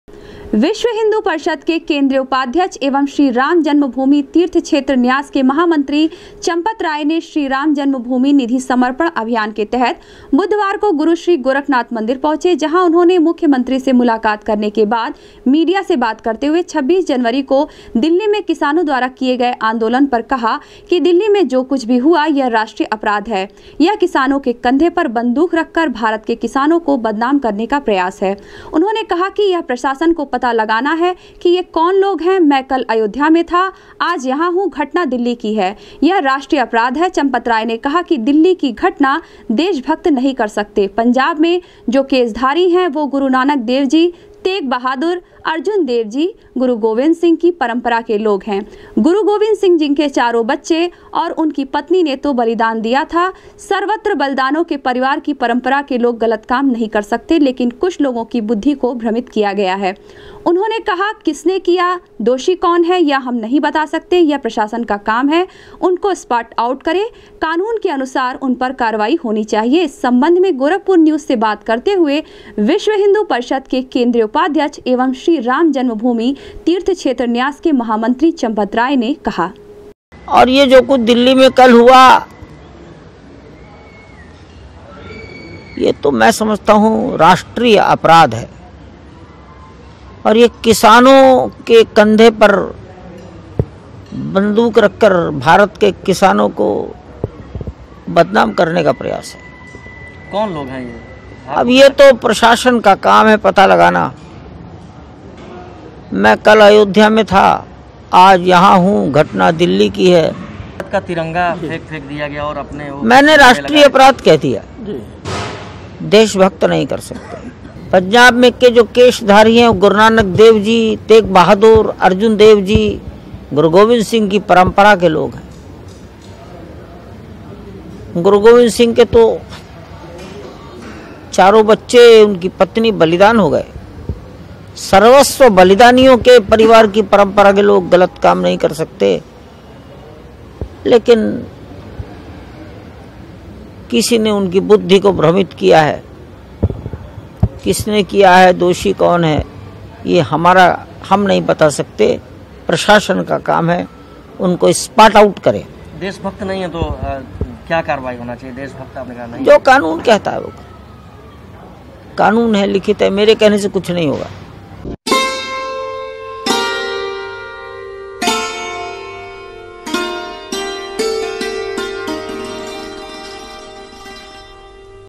विश्व हिंदू परिषद के केंद्रीय उपाध्यक्ष एवं श्रीराम जन्मभूमि तीर्थ क्षेत्र न्यास के महामंत्री चंपत
विश्व हिंदू परिषद के केंद्रीय उपाध्यक्ष एवं श्रीराम जन्मभूमि तीर्थ क्षेत्र न्यास के महामंत्री चंपत राय ने कहा,किसान आंदोलन के नाम पर गणतंत्र दिवस पर दिल्ली में जो हुआ वह है राष्ट्रीय अपराध ,जांच कर कार्यवाही करें प्रशासन